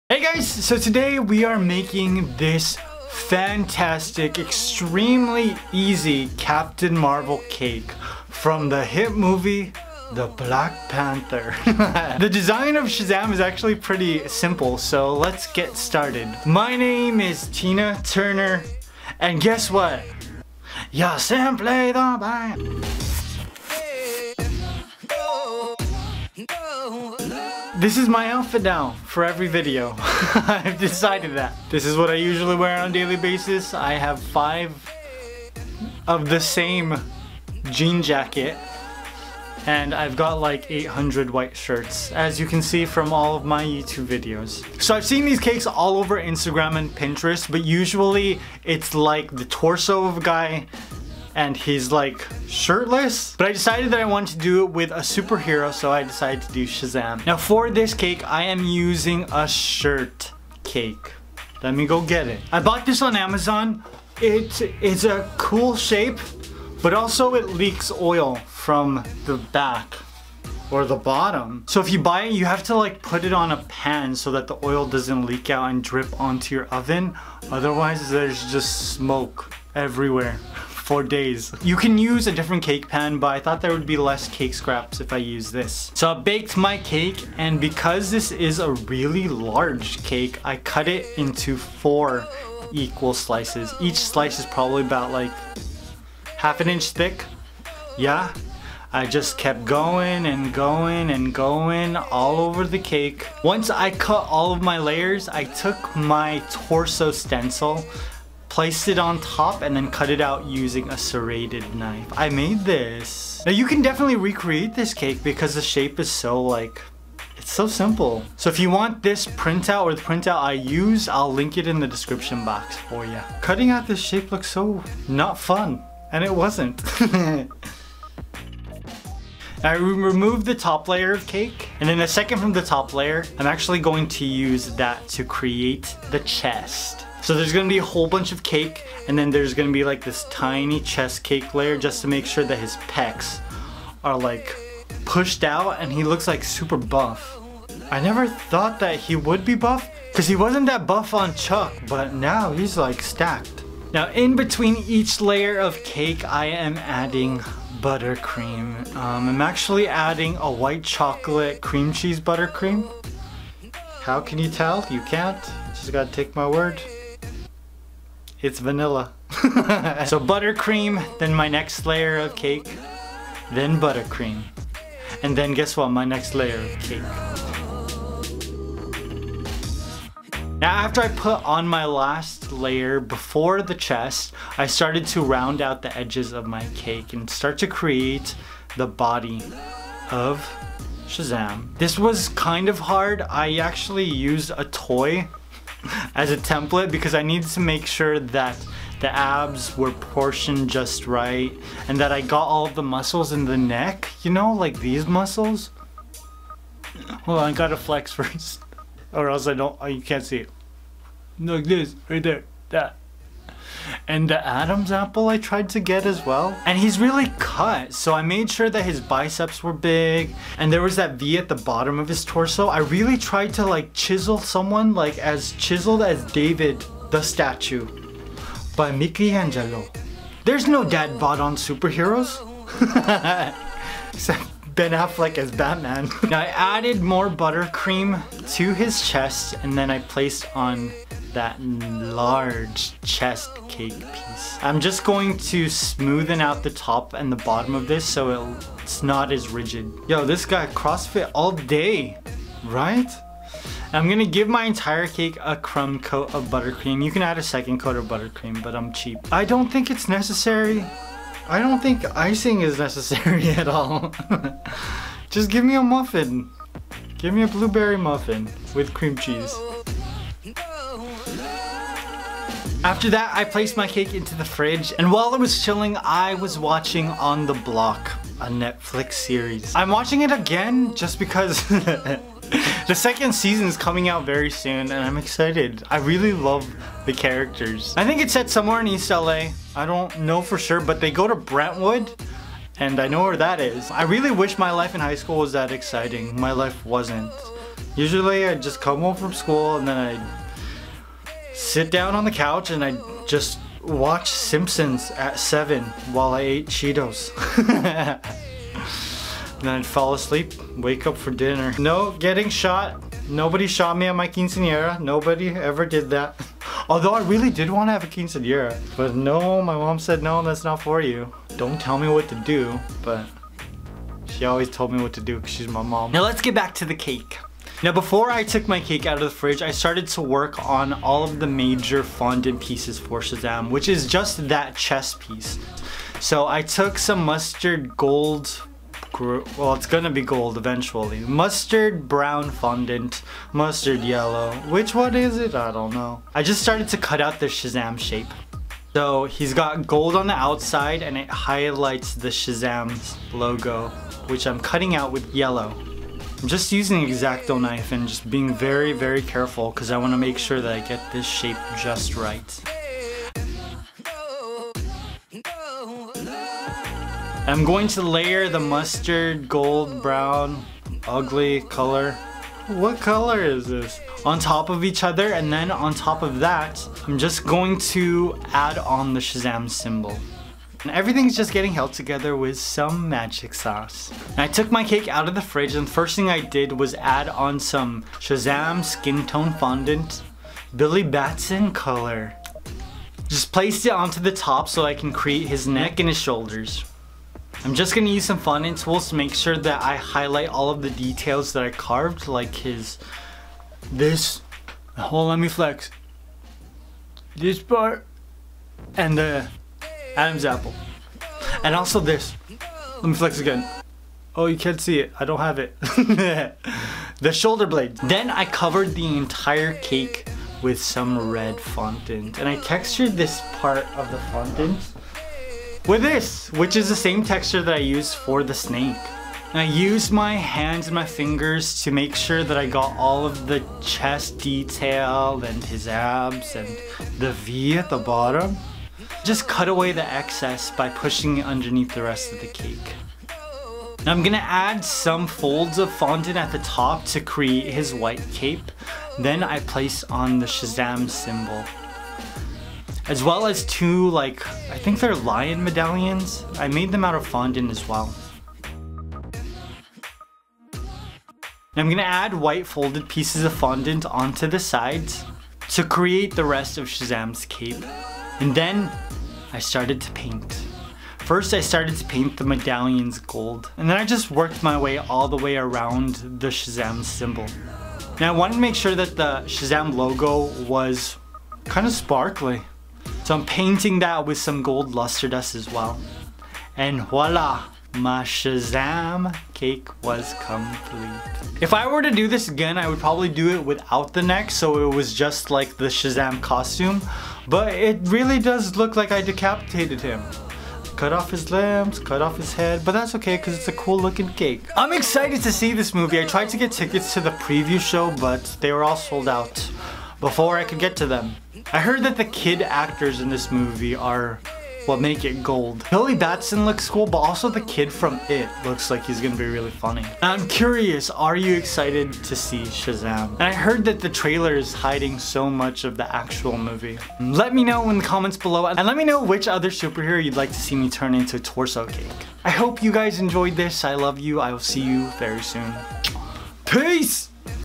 SHAZAM CAKE! | DC Justice League | Cake Art | Koalipops
Let me show you how to make this Shazam SuperHero  Cake. This is a great idea for a Birthday Party.

Can I decorate a                     Cake in under 30 min?